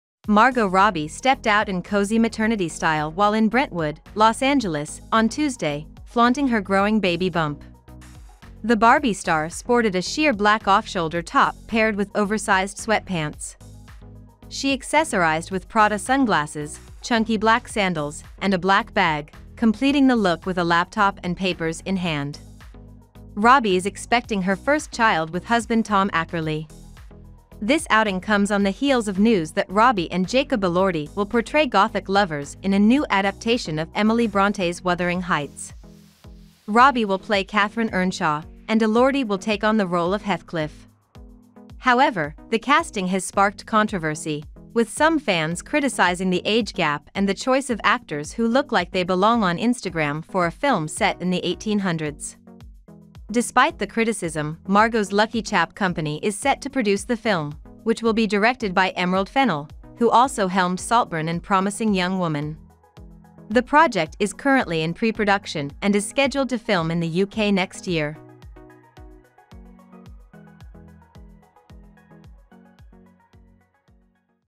‘Margot Is a Lovely and Good Actres’: Margot Robbie Shows off Her Growing Baby Bump Wile Out in LA
• Promising Young Woman

Books mentioned in this video:
• Wuthering Heights